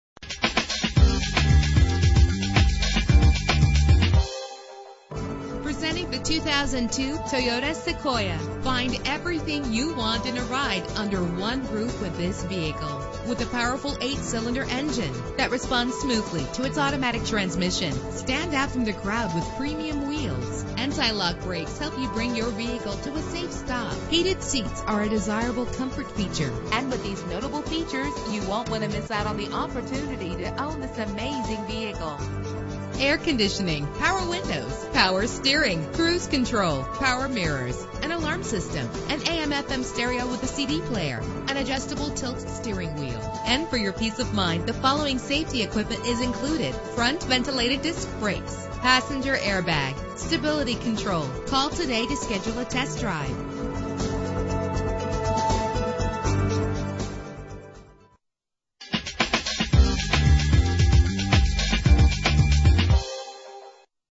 2002 Toyota Sequoia - Grandberry PA
Year: 2002
Make: Toyota
Model: Sequoia
Trim: Limited
Engine: 4.7 liter 8 cylinder 32 valve
Transmission: 4-Speed Automatic
Color: Black
Mileage: 128994
Address:
7082 US 322
Grandberry, PA 16319